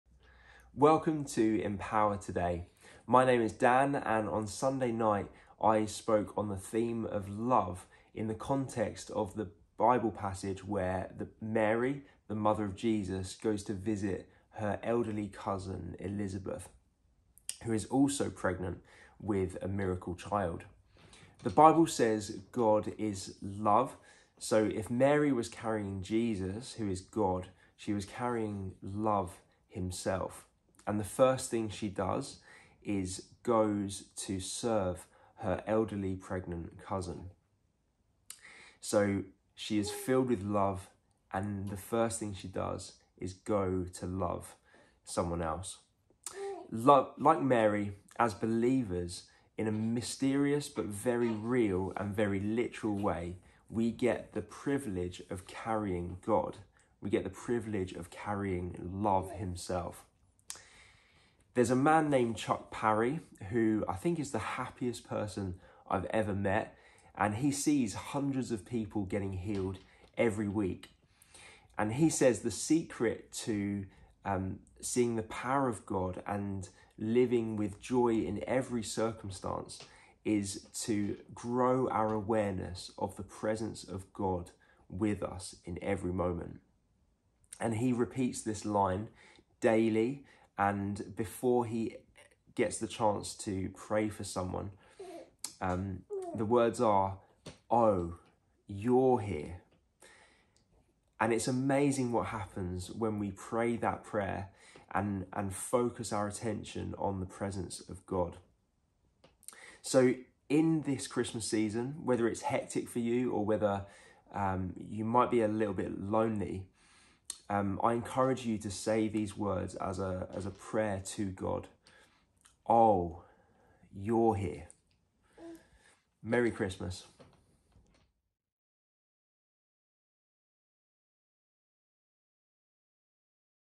God is Love // Empower 2 Day // 21 December 2023
Tonbridge Baptist Church's midweek thought from Dan Potter.

Want to find out more? We'd love to hear from you!